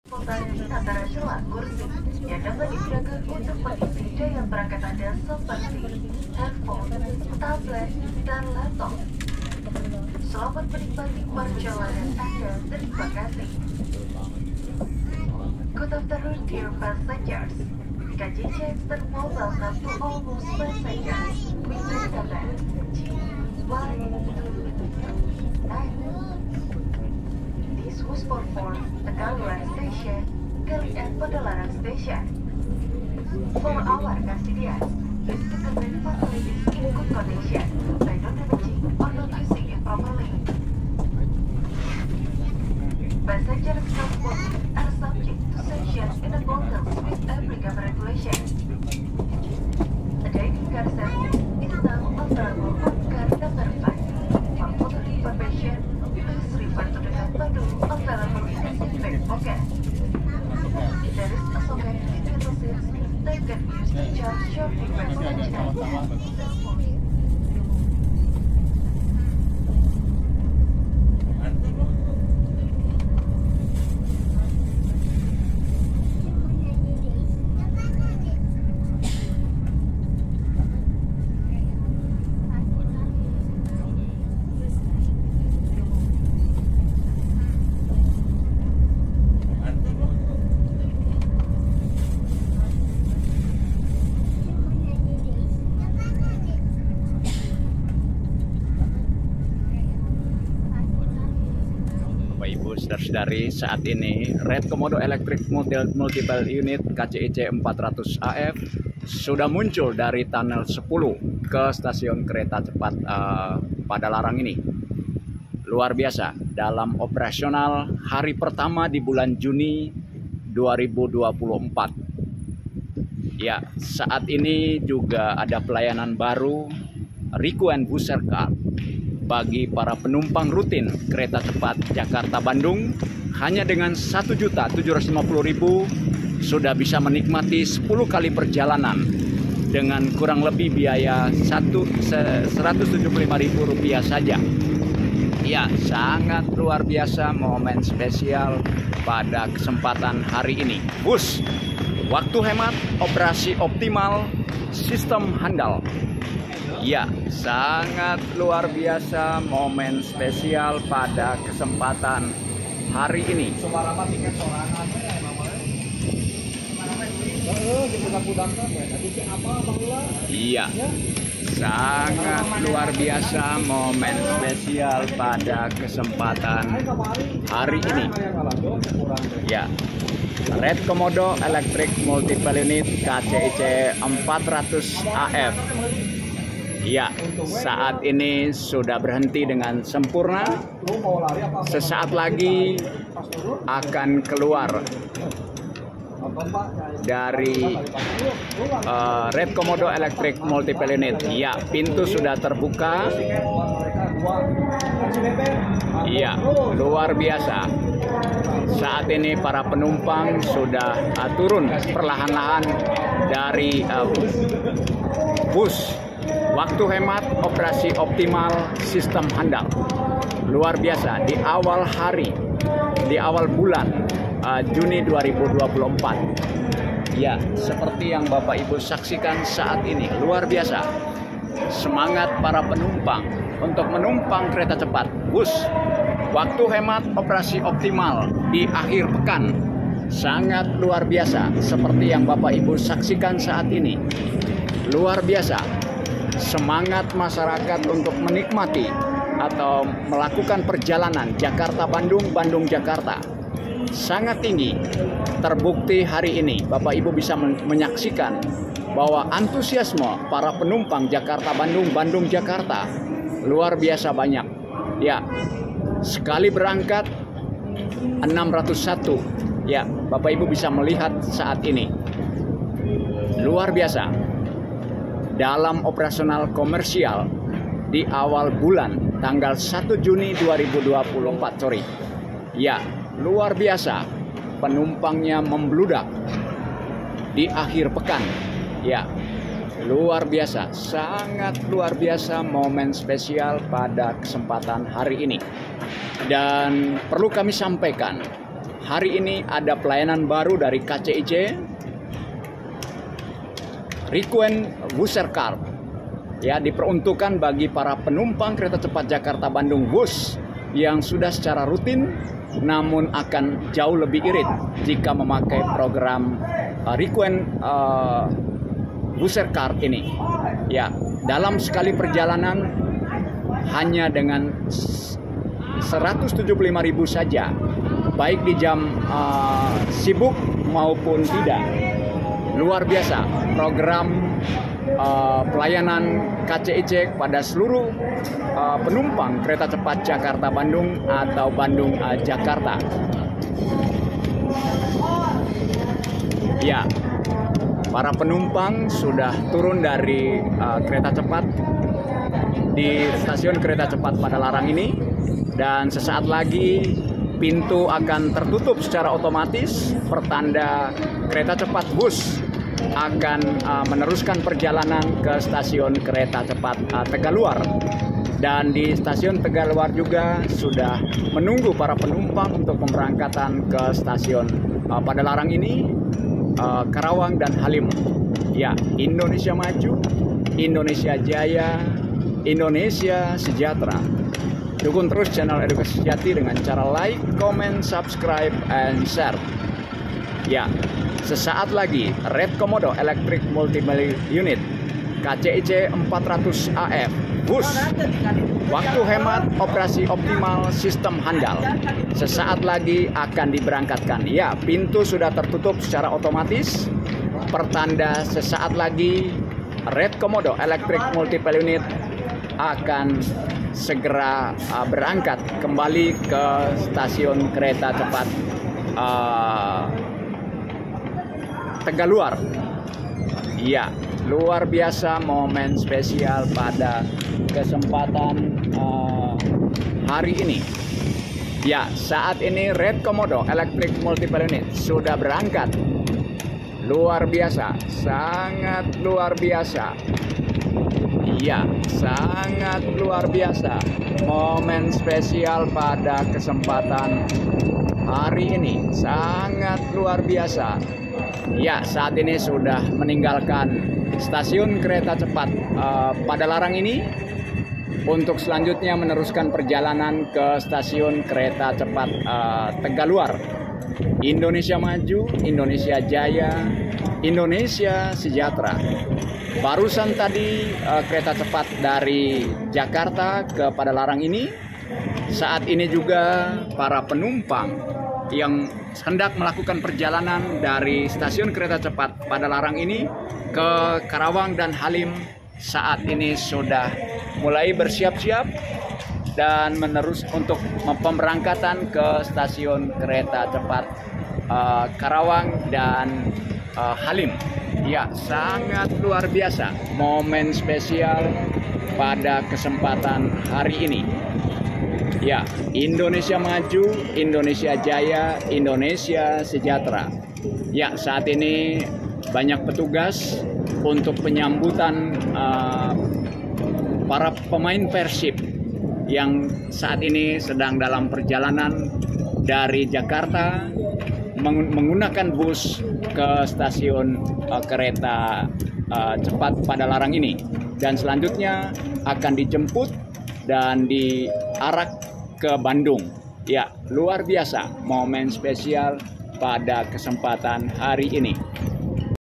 MENUMPANG WHOOSH KCJB AMAN NYAMAN DAN CEPAT SAMPAI TUJUAN@channeledukasisejati
Menumpang WHOOSH KCJB Aman Nyaman Dan Cepat Sampai Tujuan. Pada Kecepatan 350 Koin Aja Aman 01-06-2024